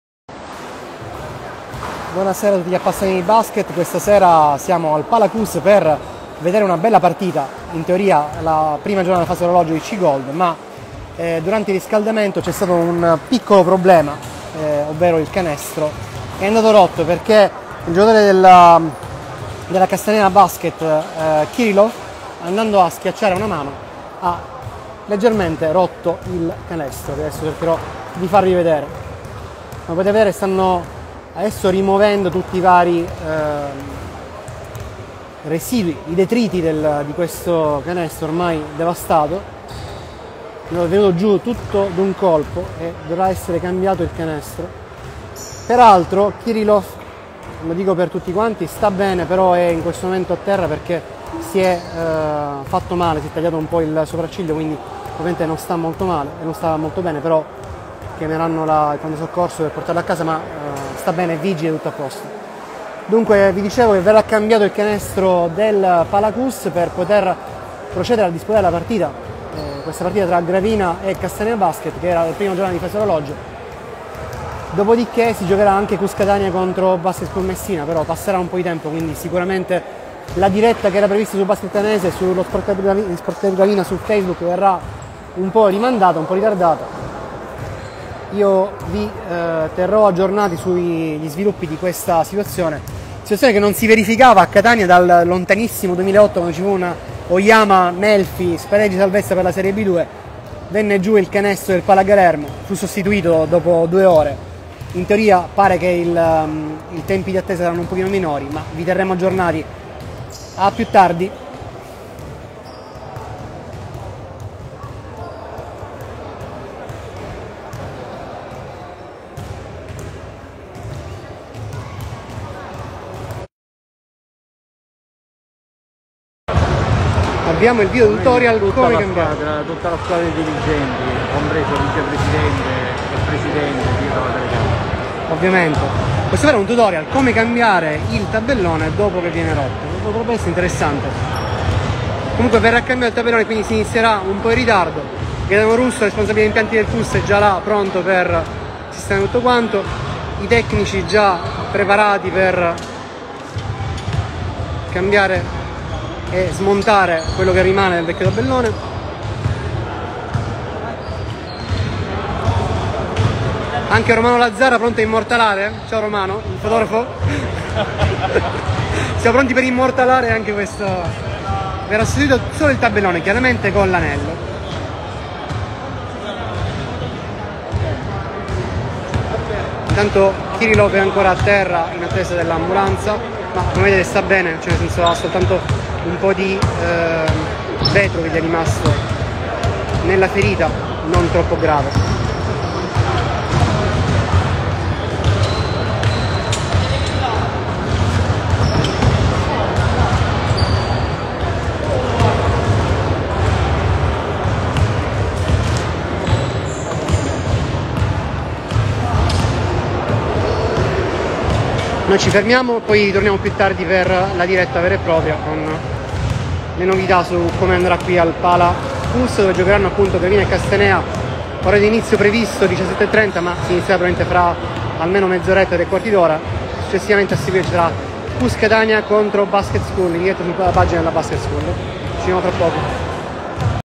La rottura del tabellone del PalaCus di Catania | 12 febbraio 2022
Il commento dopo la rottura del tabellone del PalaCus di Catania e la sua immediata sostituzione, prima di Gravina-Castanea 74-62, 15ª giornata di C Gold Fathallah, 12 febbraio 2022